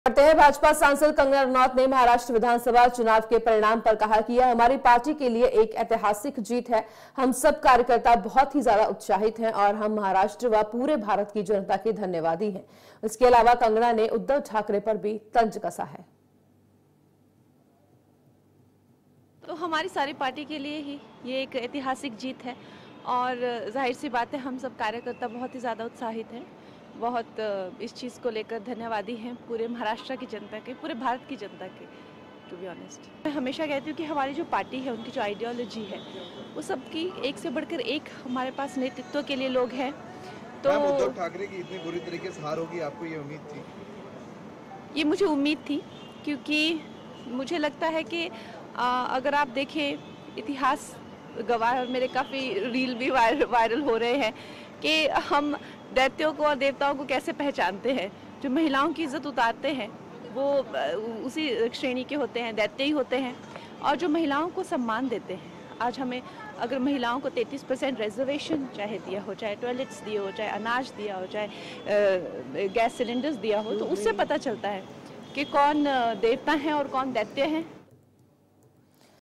Kangana Ranaut : महाराष्ट्र की जनता का किया धन्यवाद | India News Himachal
भाजपा सांसद कंगना रनौत ने महाराष्ट्र विधानसभा चुनाव के परिणाम पर कहा कि. "ये हमारी पार्टी के लिए एक ऐतिहासिक जीत है. हम सब कार्यकर्ता बहुत ही ज्यादा उत्साहित हैं और हम महाराष्ट्र व पूरे भारत की जनता के धन्यवादी हैं. इसके अलावा कंगना ने उद्धव ठाकरे पर भी तंज कसा